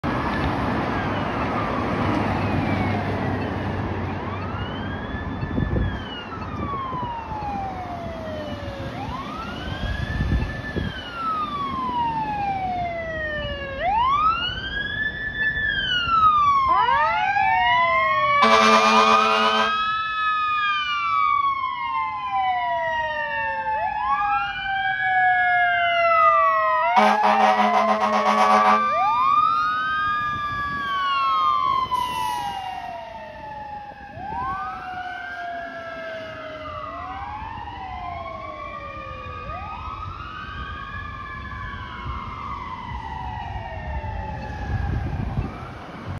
(Lots Of Horn) OCFA Engine 58 Responding
ORCO Engine 58 Responding To A Traffic Collision

Huge Thanks to all the crew members for the waves and horn